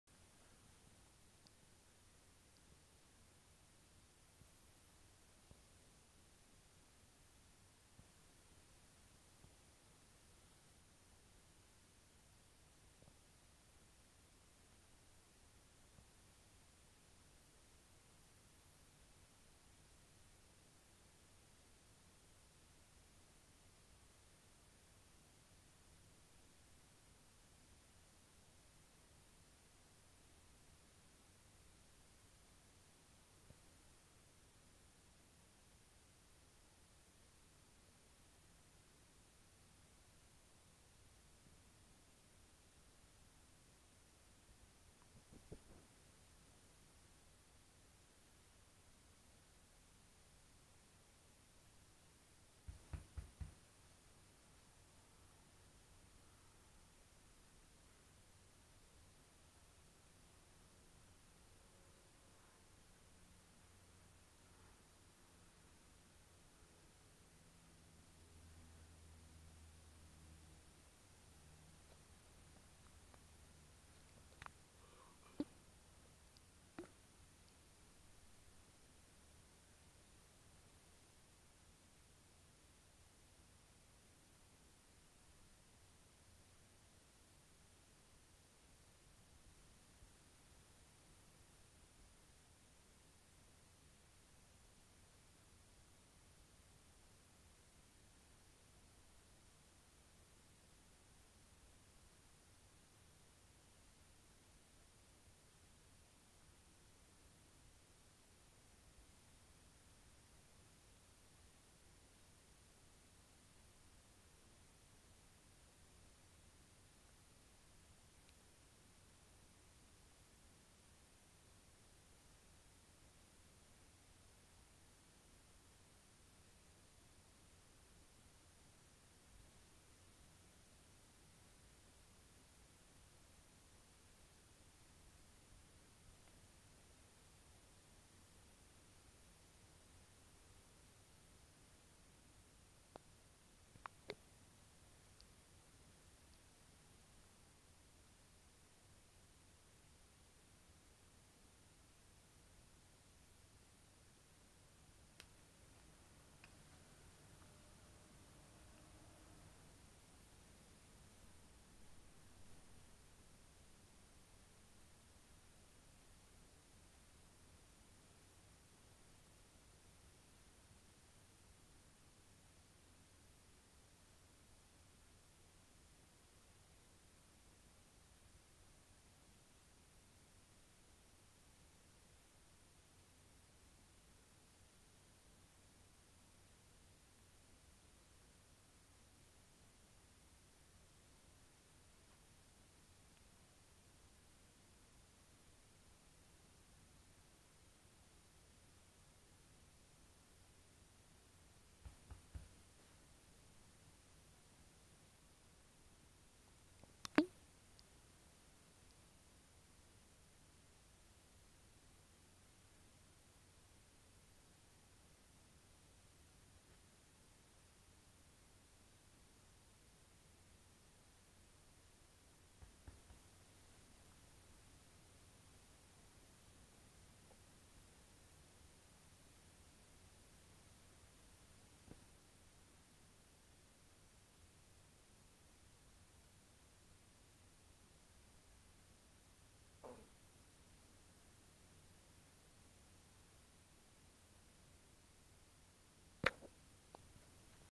Virtual Dog - Petting A Golden Retriever Up Close For 4 Minutes - SJCam SJ4000 Camera
Petting my English cream golden retriever for four minutes close up with a wide angle lens on a SJCam SJ4000 action camera (similar to a GoPro cam).